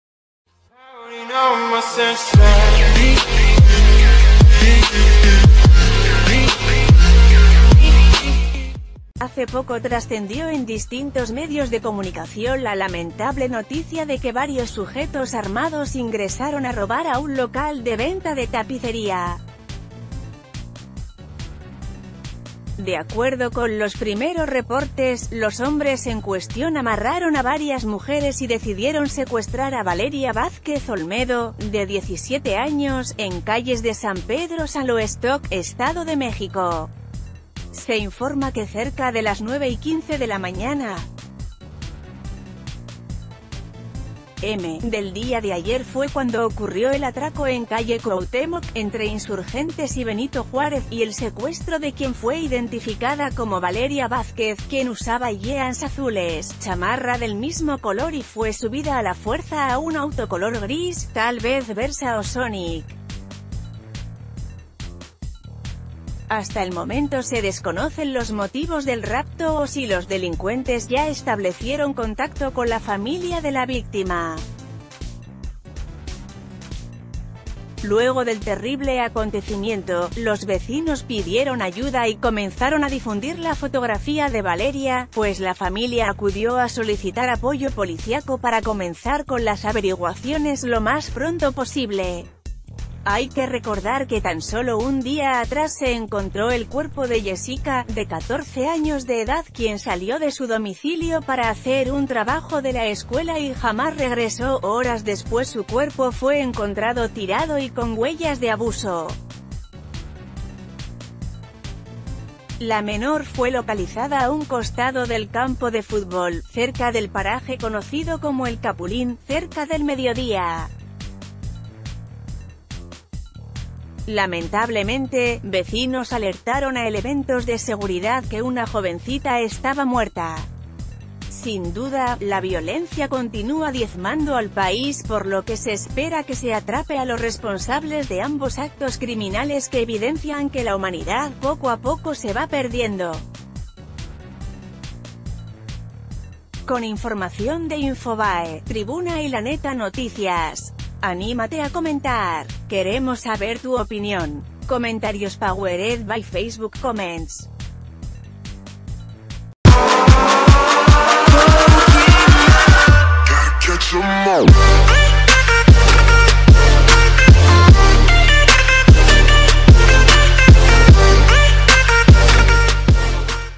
Inocente robo termina en levantón de adolescente y conmociona a todos
Hace poco trascendió en distintos medios de comunicación la lamentable noticia de que varios sujetos armados ingresaron a robar a un local de venta de tapicería. De acuerdo con los primeros reportes, los hombres en cuestión amarraron a varias mujeres y decidieron secuestrar a Valeria Vázquez Olmedo,...